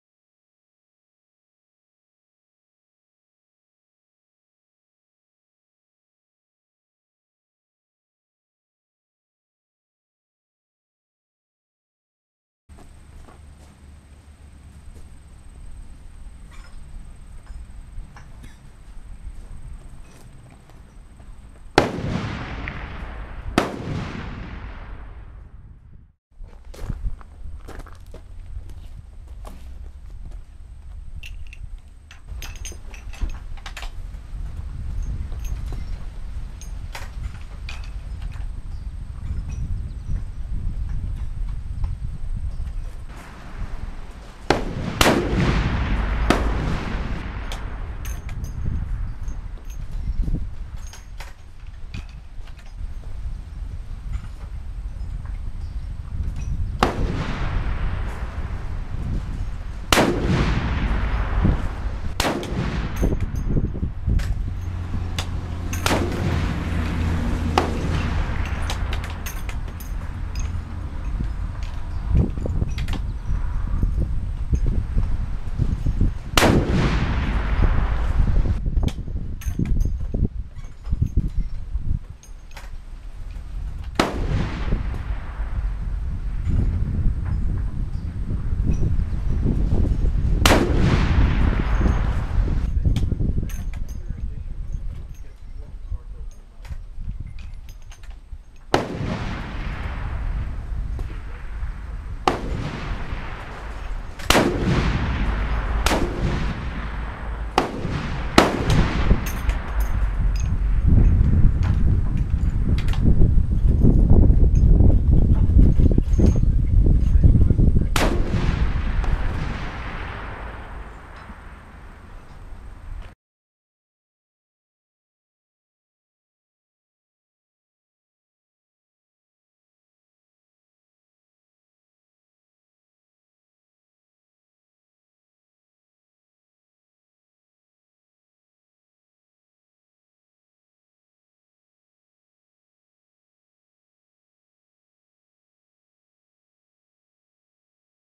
Ransom Rest 50 caliber used by FCSA World Champion
Ransom Rest 50 caliber used by FCSA World Champion Dale
This is Ransom International SL Mega Rest in use
FCSA Approved and has ben used by FCSA world Champion
 last 3 yrs in a row

Thank you to FCSA
and a special thanks to Dale
and all the other competitors who use
Ransom International and Ransom Rest products

visit us at Ransomrest.com